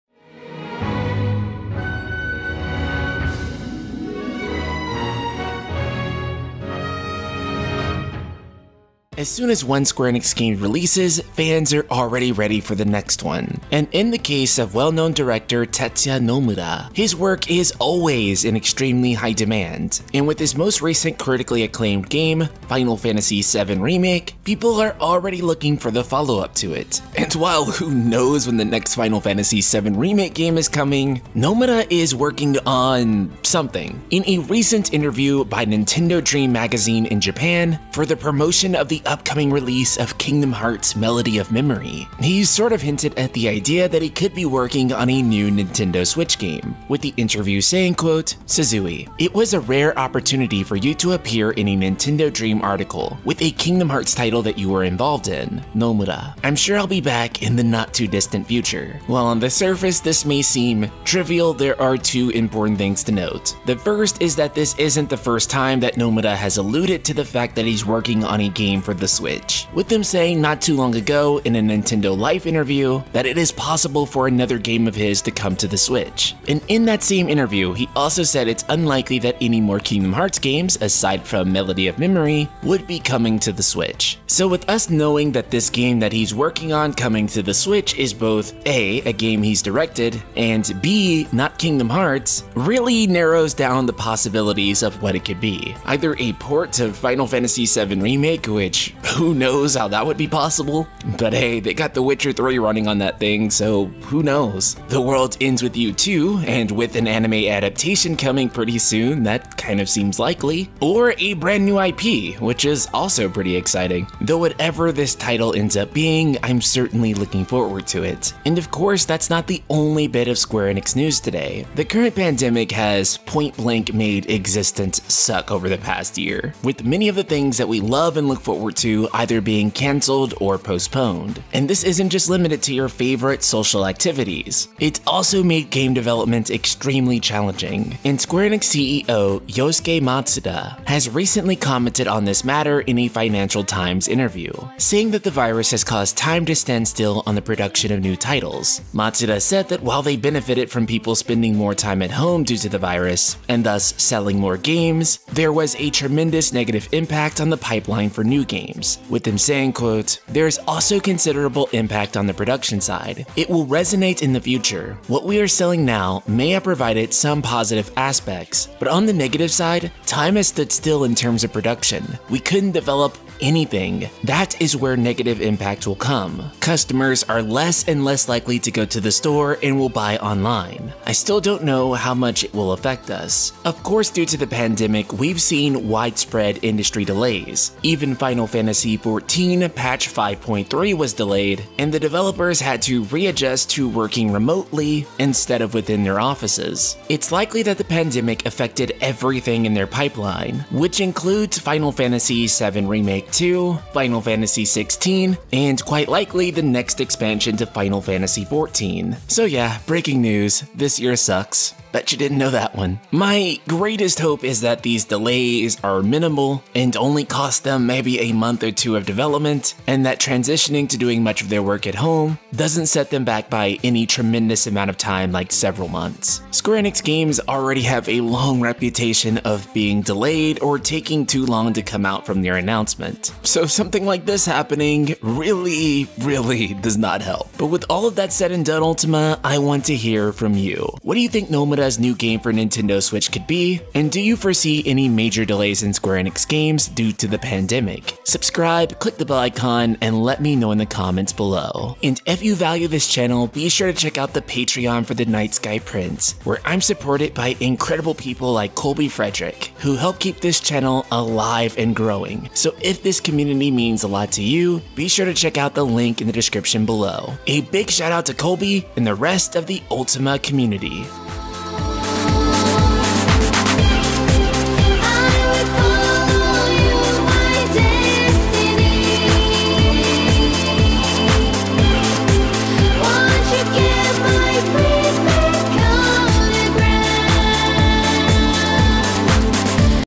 Nomura Teases New Game & Square CEO Talks Pandemic Delays
In a recent Nintendo Dream magazine, Nomura has once again alluded to the fact that he is working on a new Switch game and Square CEO Yosuke Matsuda talks how the pandemic has halted the development of multiple projects.

Ultima-Chan Artist IG: @Casas